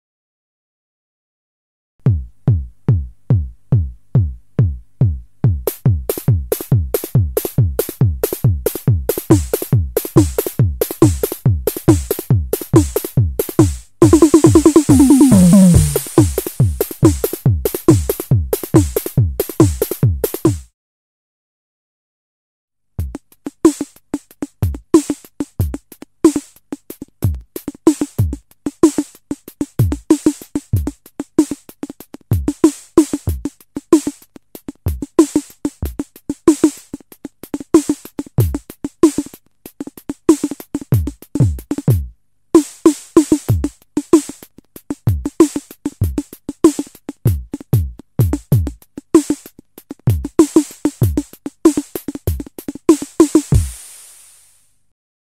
Барабаны Марш
Демонстрация электронных барабанов "Марш". Полный обзор читайте в моем блоге